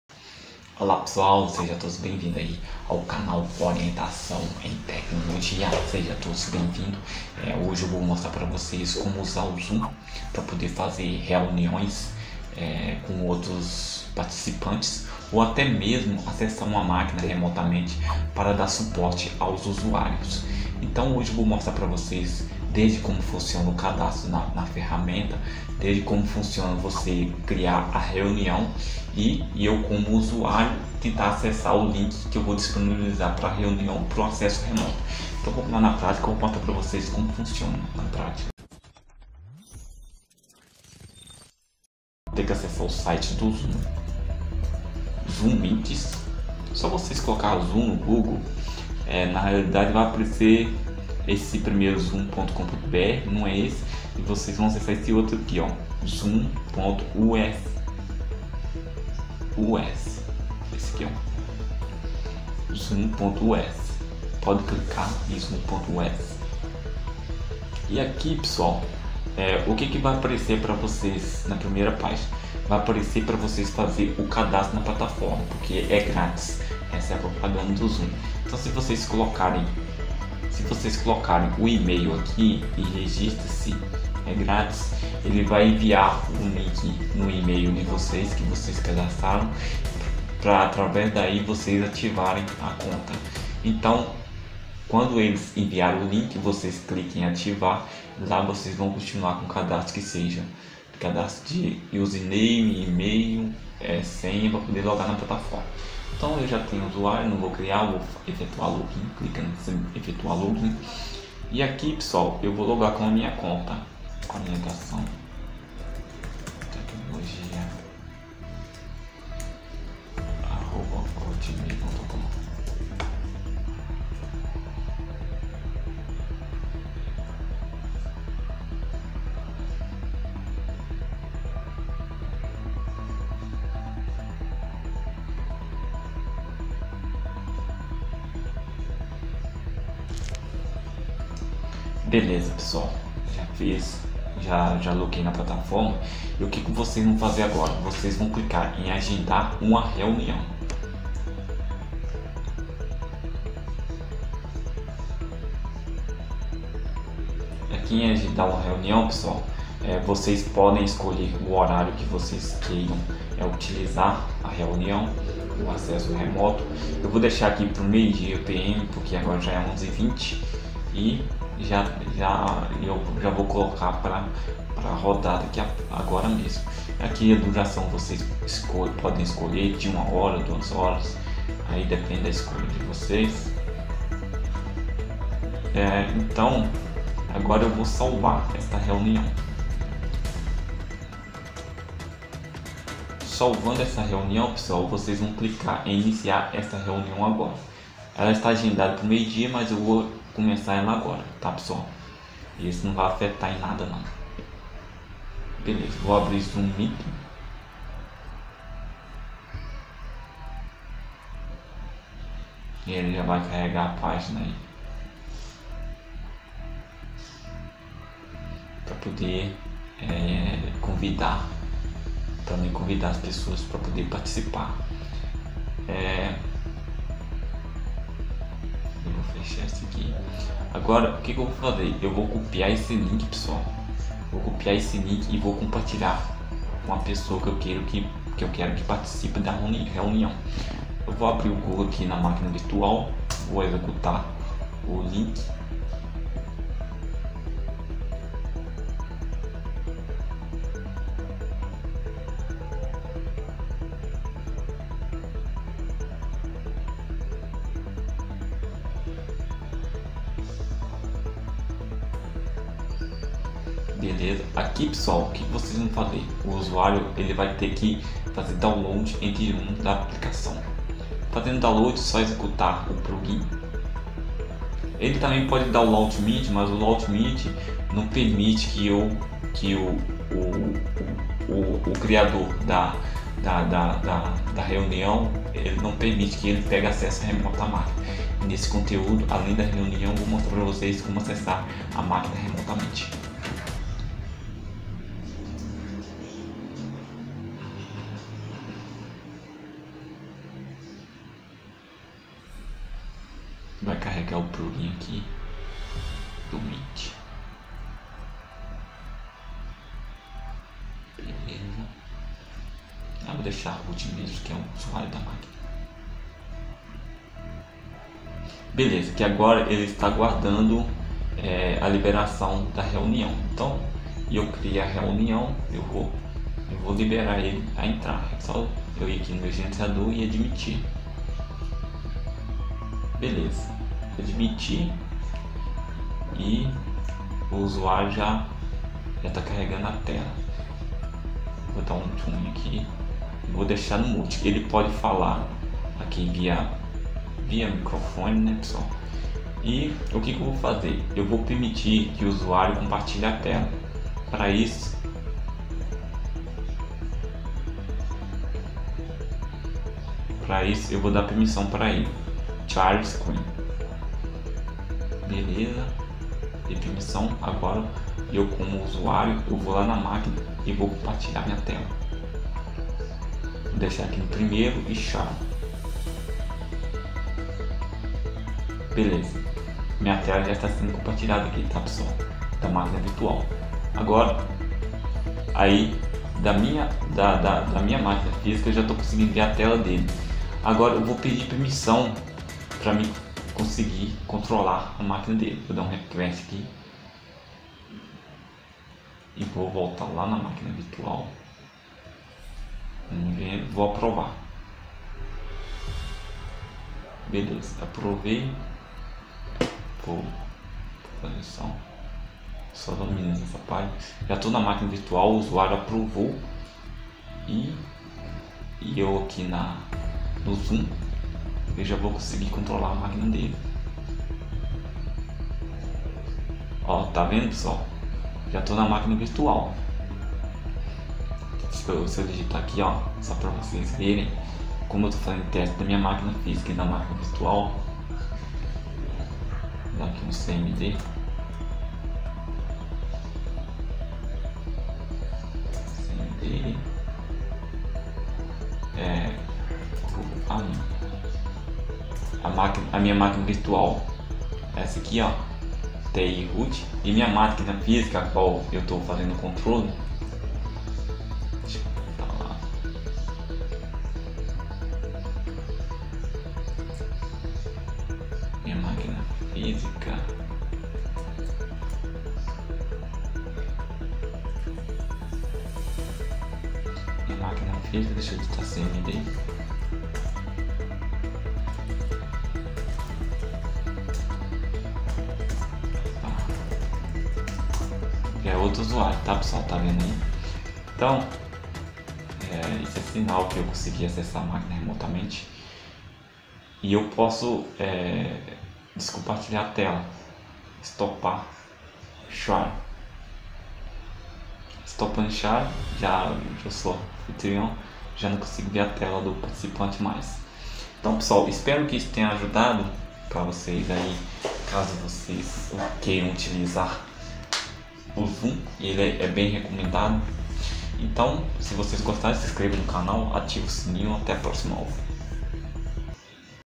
[ZOOM] COMO FUNCIONA????? - ZOOM MEETING
O Zoom Meetings é uma ferramenta de videoconferência voltada para ambientes corporativos que suporta reuniões com até 500 participantes e 10 mil expectadores no modo webinar.

COMO FUNCIONA O ZOOM MEETINGS?

Neste tutorial vou esta te mostrando COMO FUNCIONA O ZOOM MEETINGS, Vou ensinar desde o cadastro até a criação de VIDEOCONFERÊNCIA com o ZOOM.

O ZOOM também suporte em criar sessão remota para dar suporte ao usuário.